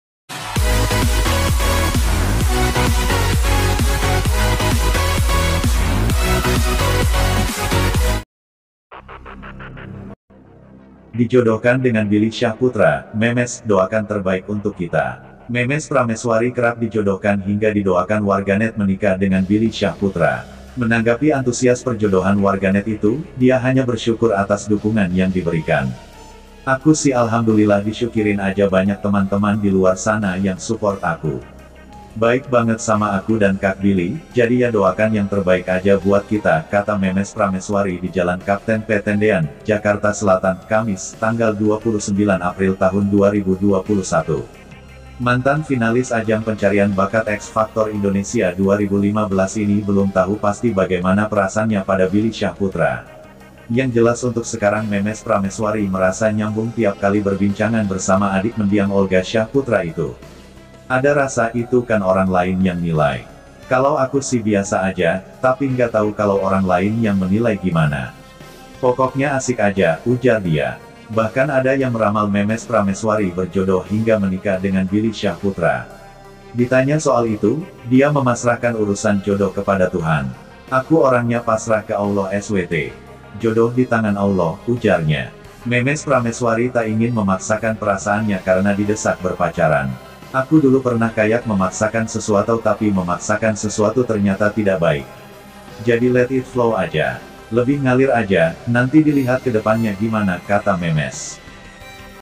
Dijodohkan dengan Billy Syahputra, Memes : Doakan Terbaik untuk Kita
Memes Prameswari kerap dijodohkan hingga didoakan warganet menikah dengan Billy Syahputra. Menanggapi antusias perjodohan warganet itu, dia hanya bersyukur atas dukungan yang diberikan.

"Aku sih alhamdulillah disyukirin aja banyak teman-teman di luar sana yang support aku. Baik banget sama aku dan kak Billy, jadi ya doakan yang terbaik aja buat kita," kata Memes Prameswari di Jalan Kapten P Tendean, Jakarta Selatan, Kamis (29/4/2021).